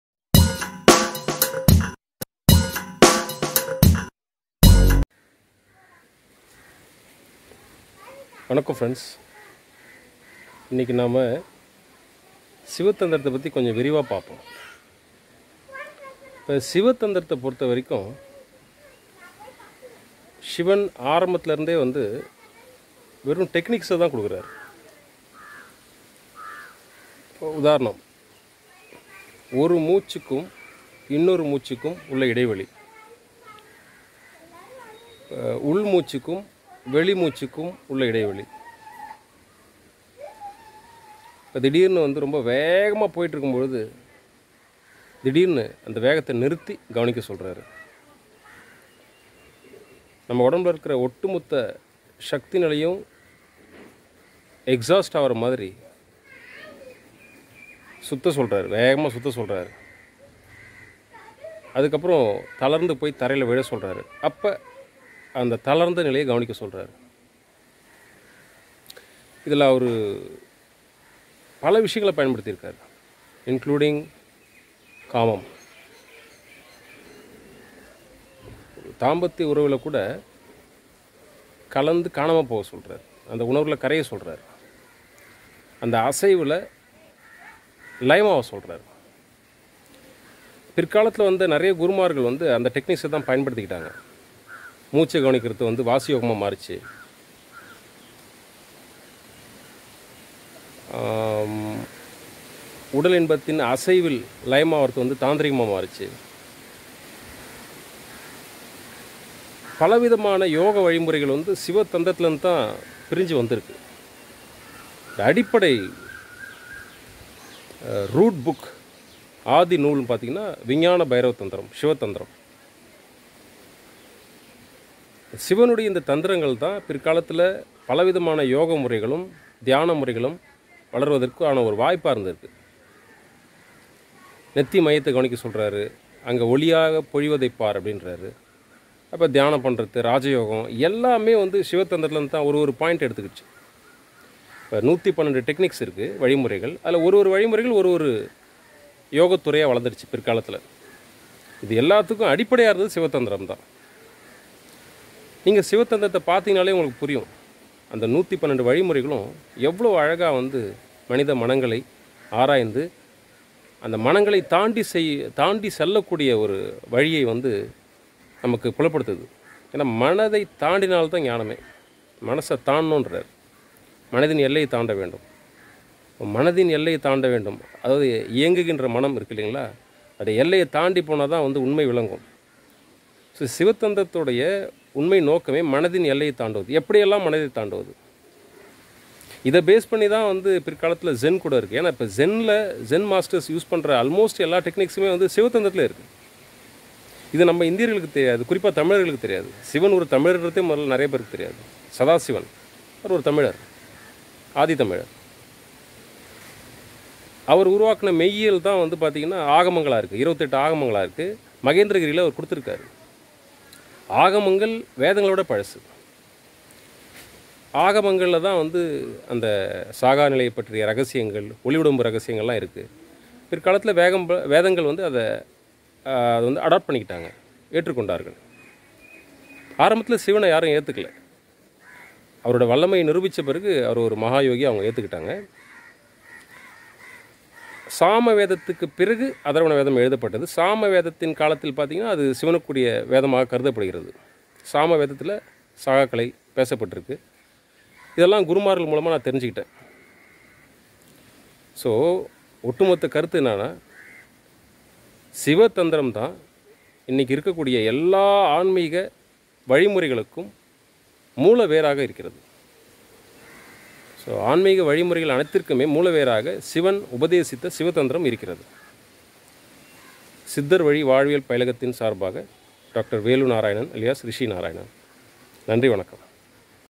சிவ தந்திரம்-Shiva tantra:the original yoga of shiva,காஷ்மீர சைவம்,part-1...
விஞ்ஞான பைரவ தந்திரம் பற்றி விளக்குகிறார் டாக்டர்.சிவ.வேல் நாராயணன்@ரிஷி நாராயணன்